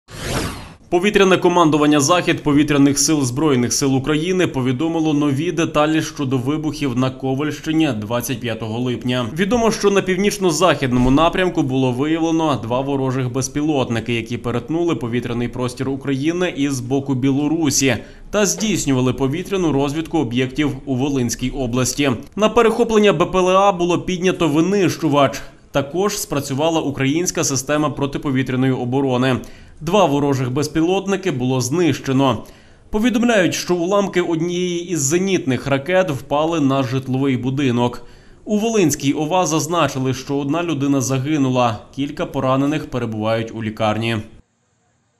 ⚡Безпілотники летіли з Білорусі, їх намагався перехопити винищувач: НОВІ ДЕТАЛІ ВИБУХІВ НА ВОЛИНІ
Повітряне командування "Захід" Повітряних Сил ЗС України повідомило нові деталі щодо вибухів на Ковельщині 25 липня.
Відомо, що на північно-західному напрямку було виявлено два ворожих безпілотники, які перетнули повітряний простір України з боку Білорусі та здійснювали повітряну розвідку об'єктів у Волинській області.
На перехоплення БПЛА було піднято винищувач, також спрацювала українська система протиповітряної оборони.
Два ворожих безпілотники було знищено.